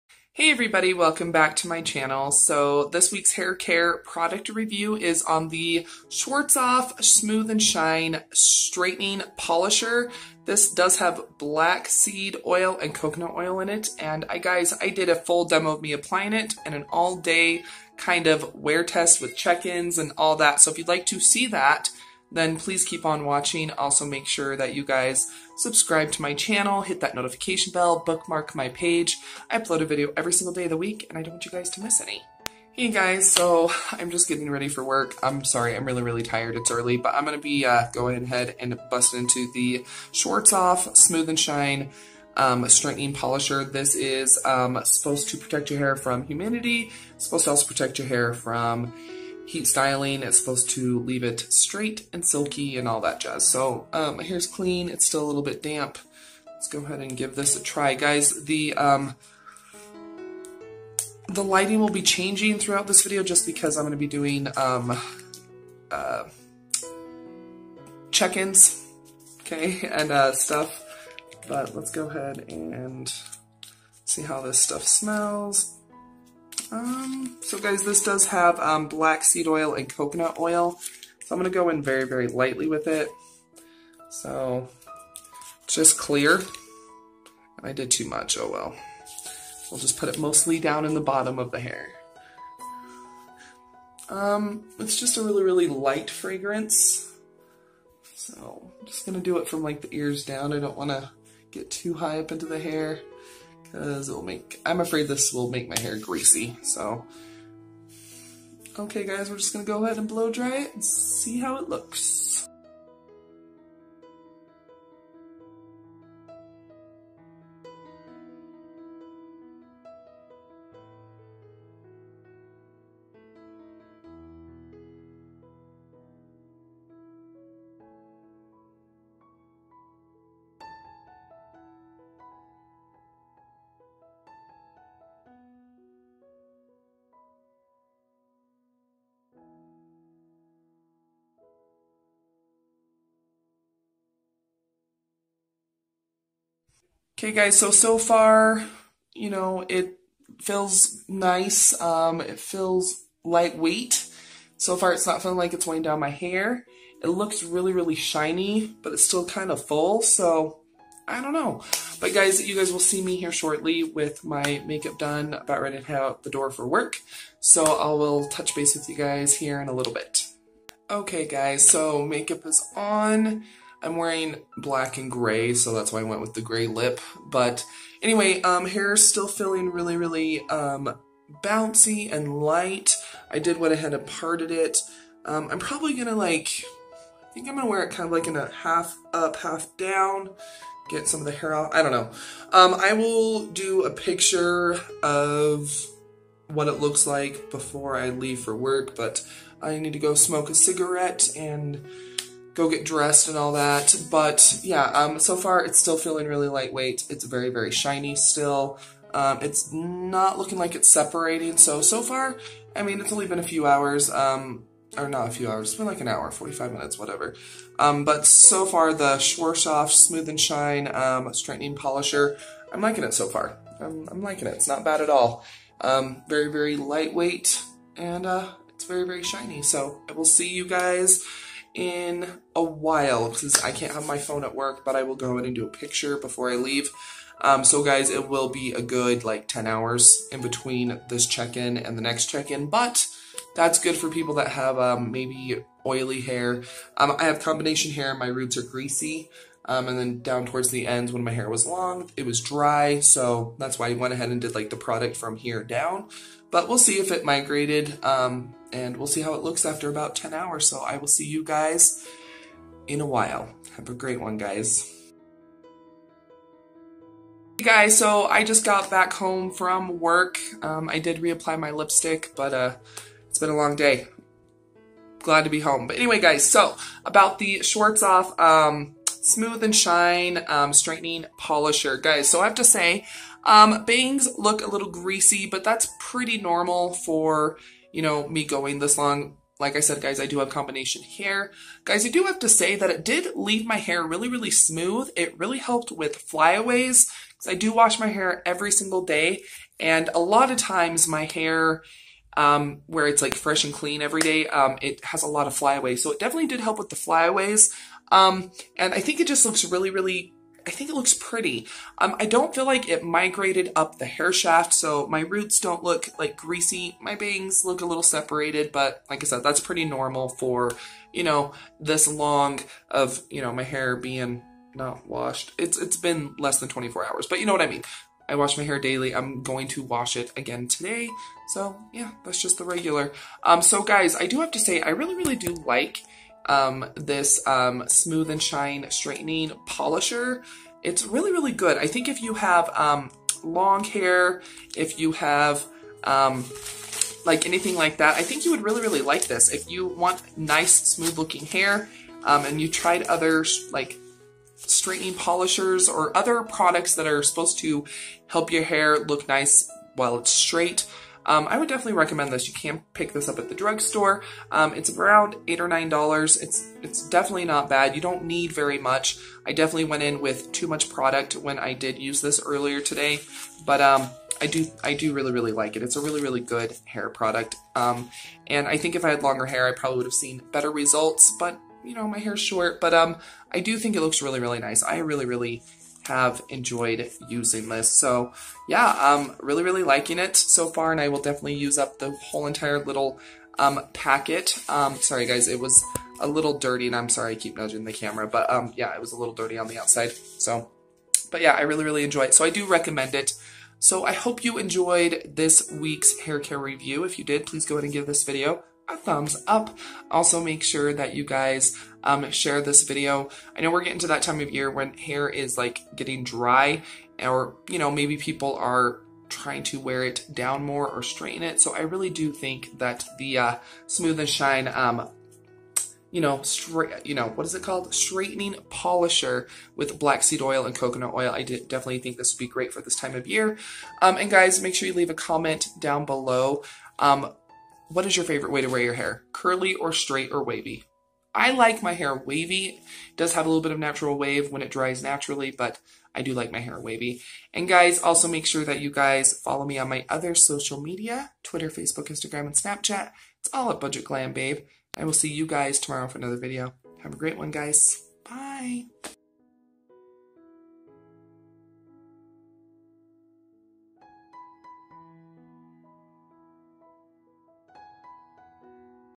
Smooth n Shine Straightening Polisher: Demo, Wear Test and Review
Smooth n Shine Straightening Polisher claims to protect hair from humidity and heat styling protection. This is a full demo and review. This is an all day try on.